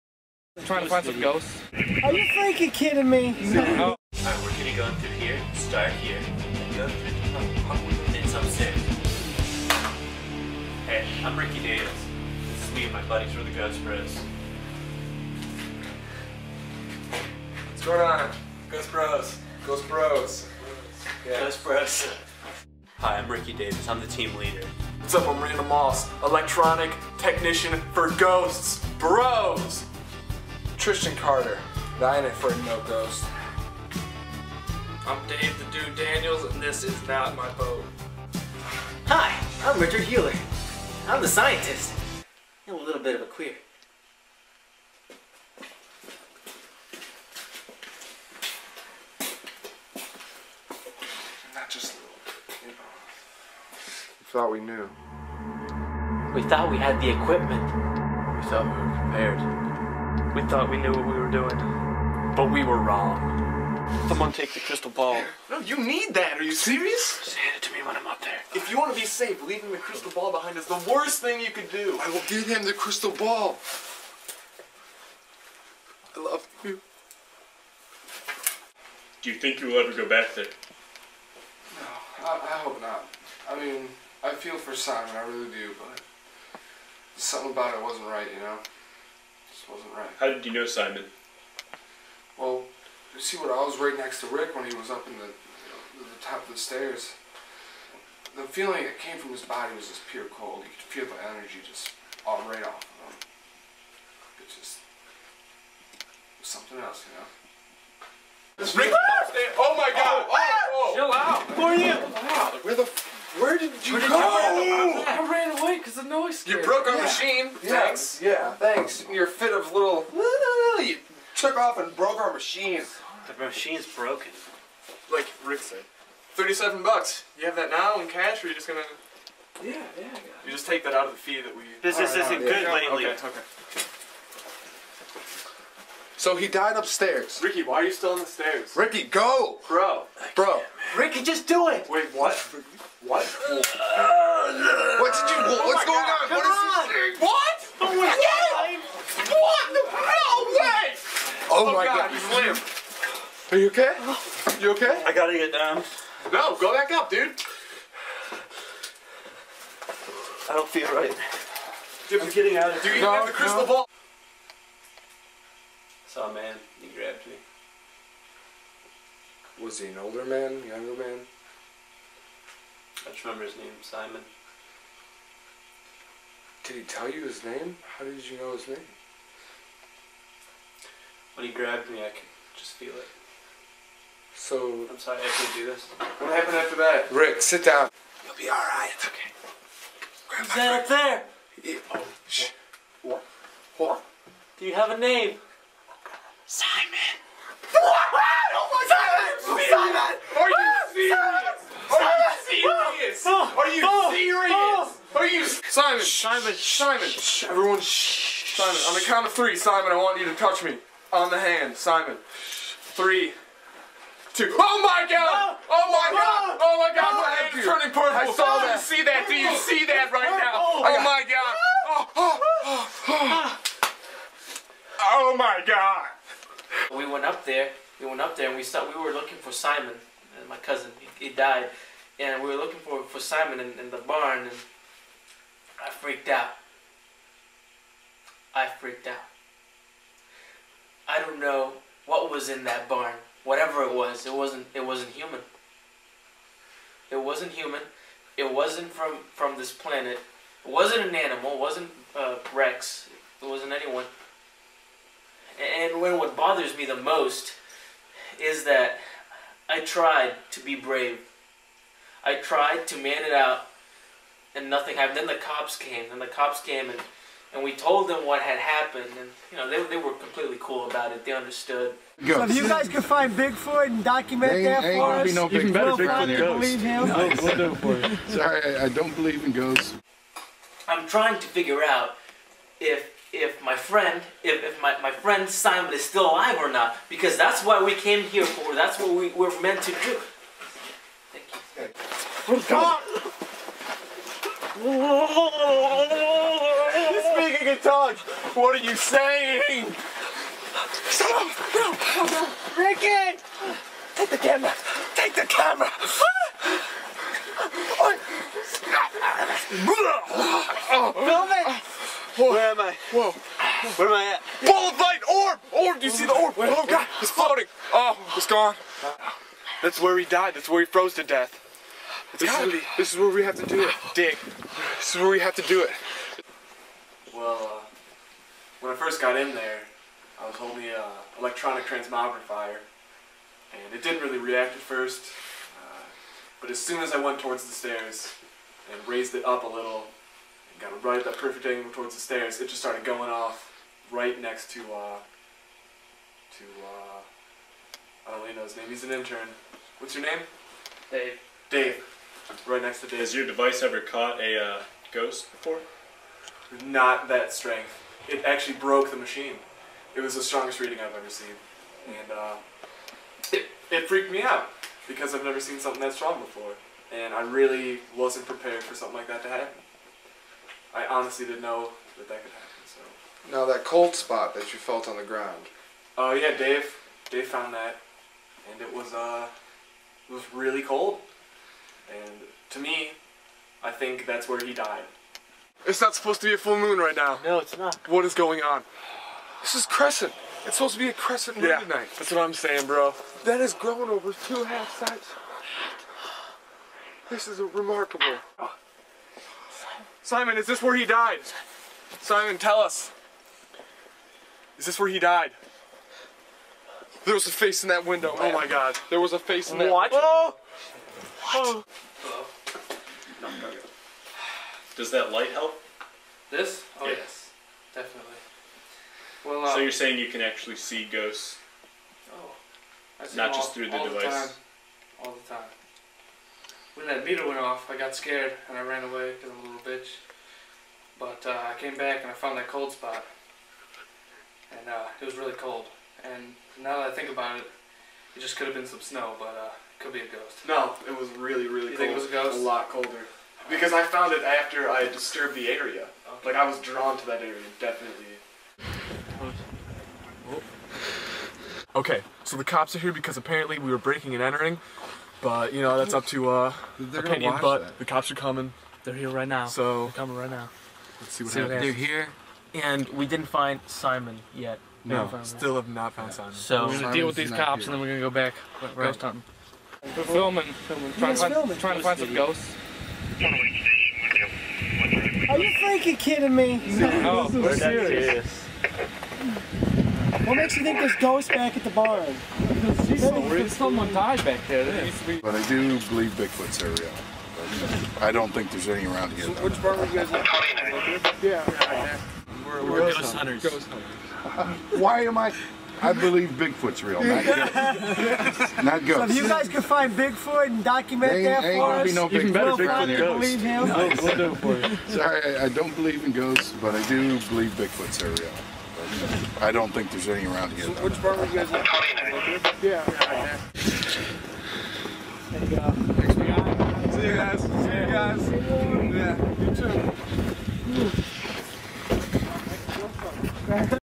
Wet Spaghetti "Ghost Bros"
Wet Spaghetti - Ghost Bros. The boys are at it again searching for ghosts! Starring Paulllfrancis as the "team leader". Spencer Pepin, Devin Staurbringer, and Mike Logan as the "investigators". Conrad Faraj as the "scientist". Filming by Meg Quisenberry. Editing by Paulllfrancis.